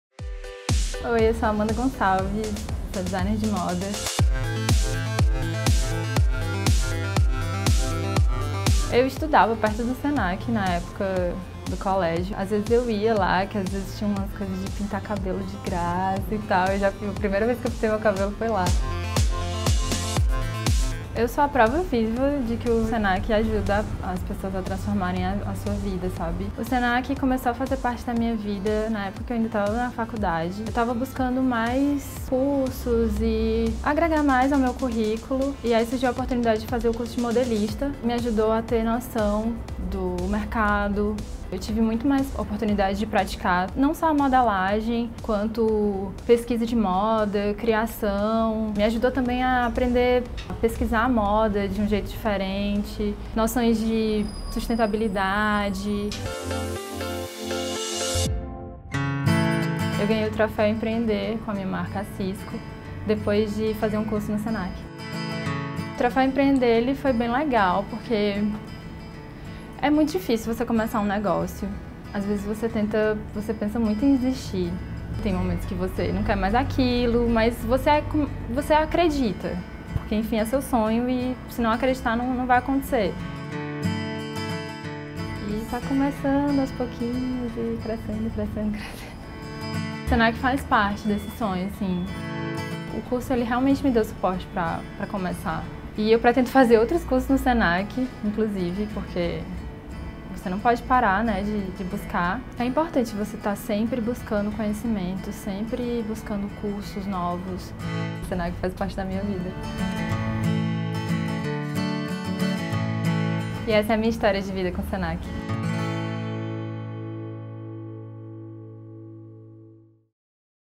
Qualificação e empreendedorismo - Amanda Gonçalves
Amanda Gonçalves é designer de moda e encontrou no Senac a oportunidade para se qualificar mais. Com o curso de modelista ela aprendeu noções do mercado, pesquisa de moda, criação e sustentabilidade. Amanda é premiada e acredita no empreendedorismo, confira no vídeo!